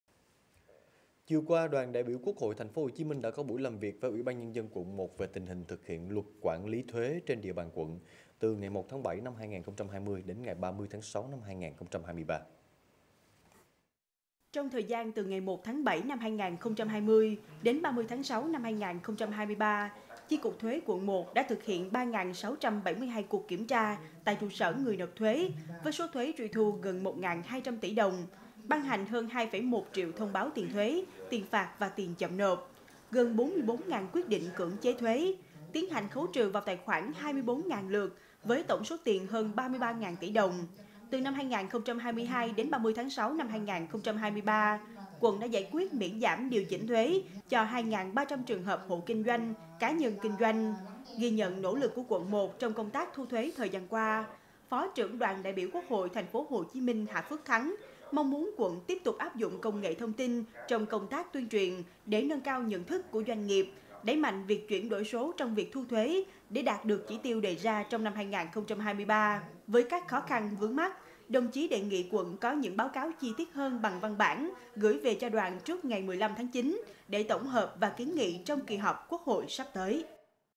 CHI CỤC THUẾ QUẬN 1 TRUY THU GẦN 1200 TỶ ĐỒNG
Chiều qua, Đoàn đại biểu quốc hội TP.HCM đã có buổi làm việc với UBND quận 1 về tình hình thực hiện Luật quản lý thuế trên địa bàn quận từ 1/7/2020 đến 30/6/2023.